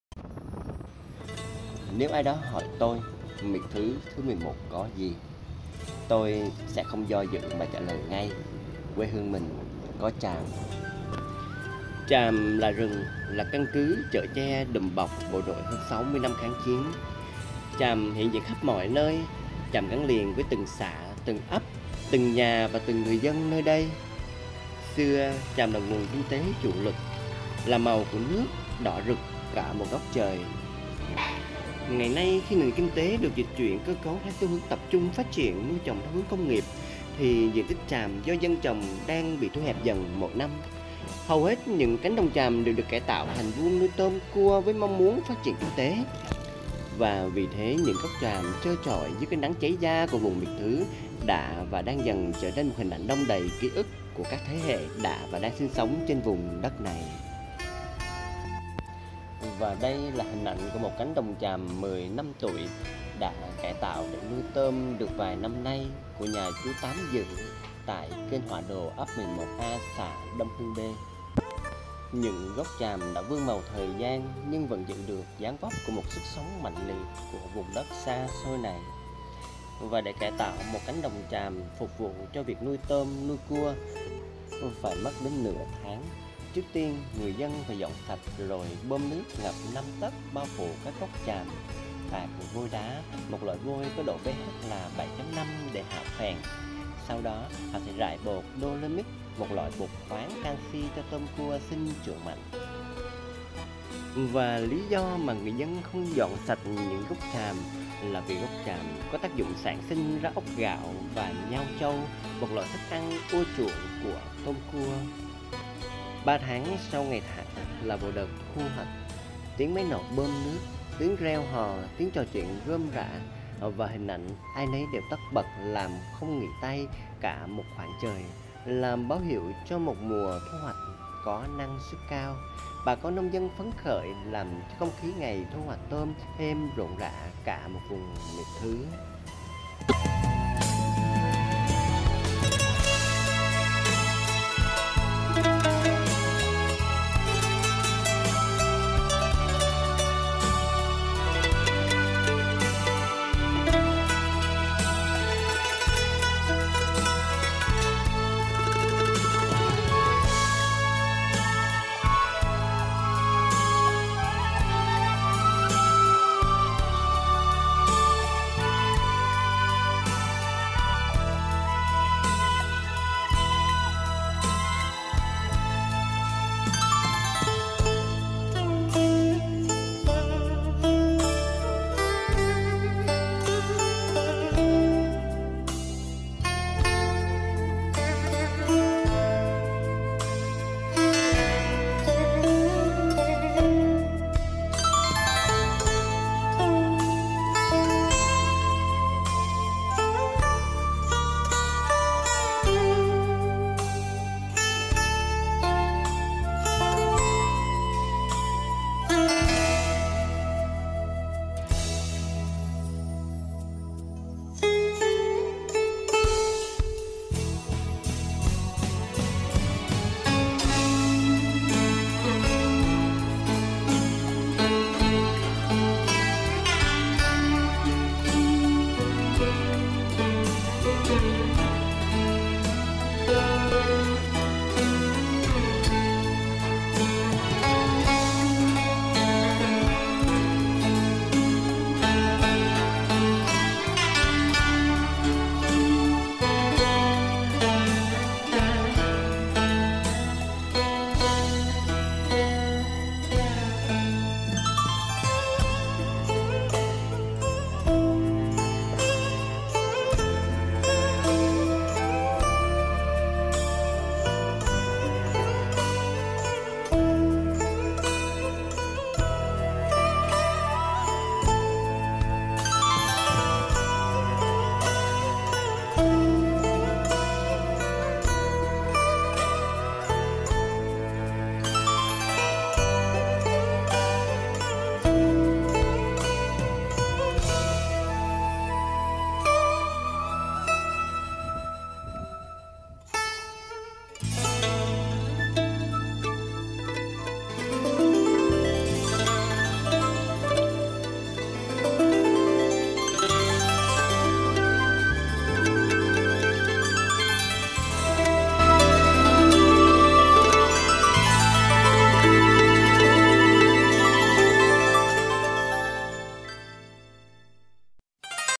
👉 KHÁM PHÁ ➸ NHỮNG GỐC TRÀM Ở VUÔNG TÔM 🦐 Ở VÙNG MIỆT THỨ❗
👉 KHÁM PHÁ MIỆT THỨ❗
      NHỮNG GỐC TRÀM Ở VUÔNG TÔM 🦐❗
➸ Nhà chú TÁM DỮ, Ấp
Xã

     ① - Nếu ai đó hỏi tôi: Miệt Thứ Thứ 11 có gì❓
            Tôi sẽ không do dự mà trả lời ngay:
            ➸ ❛Quê hương mình có Tràm❜

     ② - Tràm là rừng, là căn cứ chở che, đùm bọc bộ đội
            suốt hơn ⑥⓪ năm kháng chiến.
            Tràm xuất hiện khắp mọi nơi, tràm gắn liền với
            từng xã, từng ấp, từng nhà và từng người dân
            nơi đây.

      ③ - Xưa: Tràm là nguồn kinh tế chủ lực, là màu
                       của nước - đỏ rực cả ① góc trời, con kênh.

             Nay: Khi nền kinh tế được dịch chuyển sang
                       cơ cấu theo xu hướng tập trung phát triển
                       nuôi trồng theo hướng công nghiệp thì ...
                       ① phần diện tích tràm do dân trồng đang
                       bị thu hẹp dần mỗi năm.

             Hầu hết các cánh đồng tràm đã được cải tạo
             thành vuông nuôi tôm 🦐 - cua 🦀
             với mong muốn phát triển kinh tế hộ gia đình.

       ④ - Vậy nên, những GỐC TRÀM trơ trọi dưới
             cái nắng cháy da của vùng quê Miệt Thứ
             đã và đang dần trở nên ➊ hình ảnh đong đầy
             ký ức của các thế hệ đã và đang sinh sống
             trên mảnh đất này.